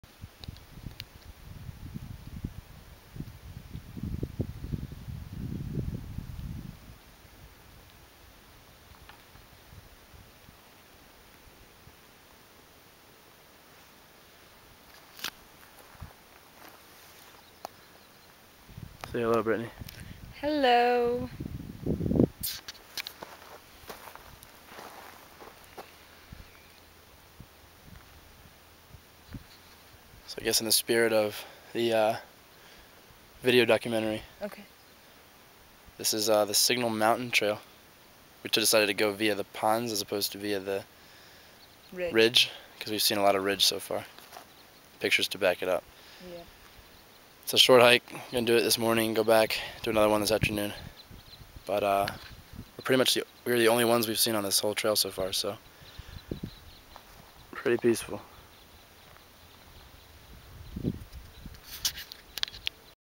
Signal Mountain Pond
A peaceful pond on the way up Signal Mountain in Grand Teton.